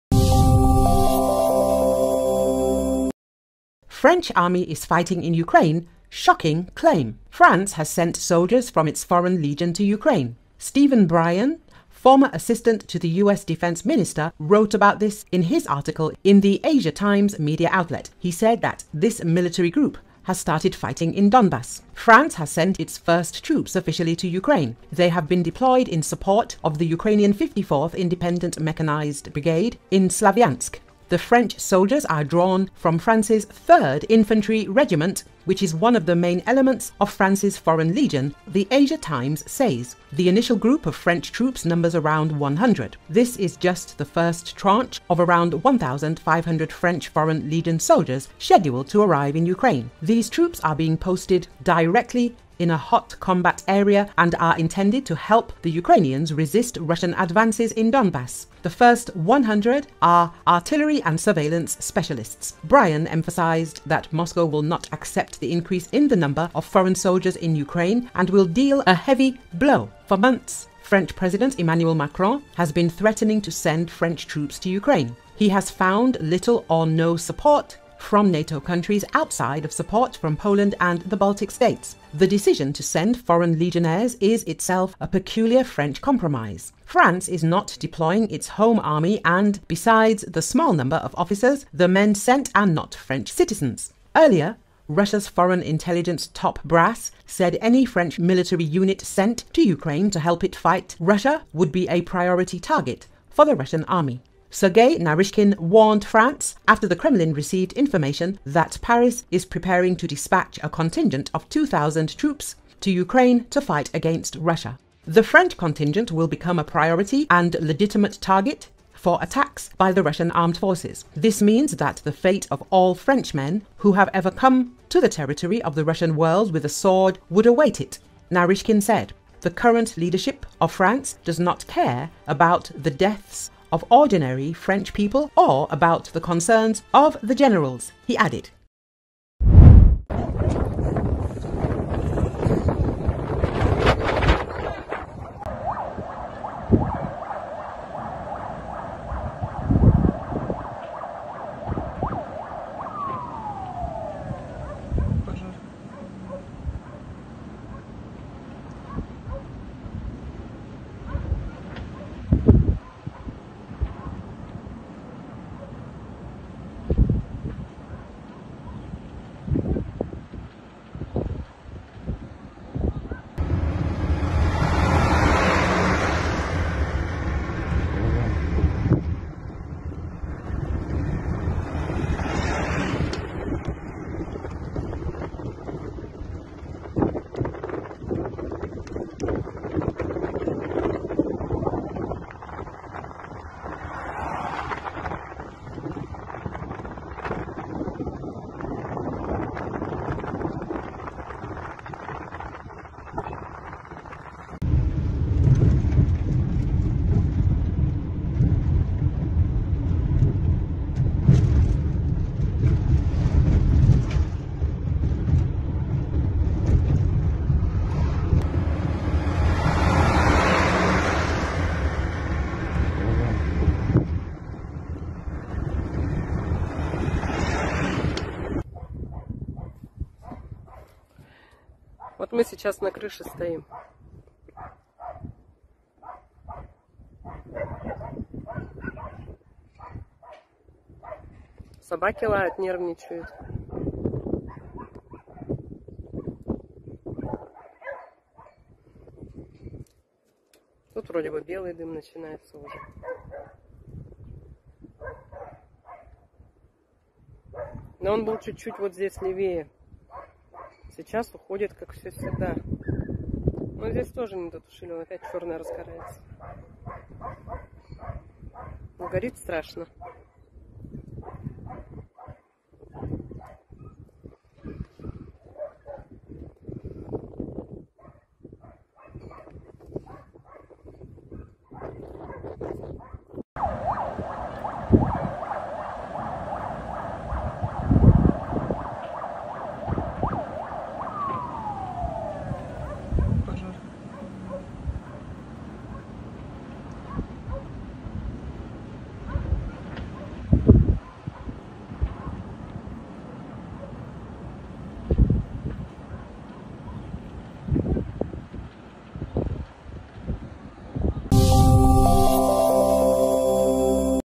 French army is fighting in Ukraine - Shocking claim
France has sent soldiers from its foreign legion to Ukraine.
Stephen Bryan, former assistant to the US defense minister, wrote about this in his article in Asia Times media outlet. He said that this military group had started fighting in Donbas.
“France has sent its first troops officially to Ukraine. They have been deployed in support of the Ukrainian 54th Independent Mechanized Brigade in Slavyansk. The French soldiers are drawn from France’s 3rd Infantry Regiment, which is one of the main elements of France’s Foreign Legion”, Asia Times says.
The initial group of French troops numbers around 100. This is just the first tranche of around 1,500 French Foreign Legion soldiers scheduled to arrive in Ukraine.
These troops are being posted directly in a hot combat area and are intended to help the Ukrainians resist Russian advances in Donbas. The first 100 are artillery and surveillance specialists.
Brian emphasized that Moscow will not accept the increase in the number of foreign soldiers in Ukraine and will deal a heavy blow.
For months French President Emanuel Macron has been threatening to send French troops to Ukraine. He has found little or no support from NATO countries outside of support from Poland and the Baltic States.
The decision to send Foreign Legionnaires is, itself, a peculiar French compromise. France is not deploying its home army and, besides the small number of officers, the men sent are not French citizens.
Earlier, Russia's foreign intelligence top brass said any French military unit sent to Ukraine to help it fight Russia would be a "priority" target for the Russian army.
Sergey Naryshkin’s warned France after Kremlin received information that Paris is preparing to dispatch a contingent of 2,000 troops to Ukraine to fight against Russia.
"French contingent will become a priority and legitimate target for attacks by the Russian Armed Forces. This means that the fate of all Frenchmen who have ever come to the territory of the Russian world with a sword would await it," Naryshkin said.
"The current leadership of the France does not care about the deaths of ordinary French people or about the concerns of the generals”, he added.